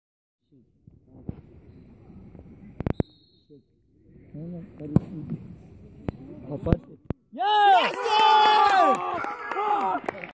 Kreisliga B5: Sehenswertes Kopfballtor
Video vom sehenswerten Kopfballtor von Martin Reichert für SV Eintracht. Szene aus dem Spiel SV Eintracht vs. Spvgg Stetten/Filder II. Vorlage Angelo Pagano. Kreisliga B5 Stuttgart, 3. Mai 2015

Eingeschickt von FuPaner Christian Neumann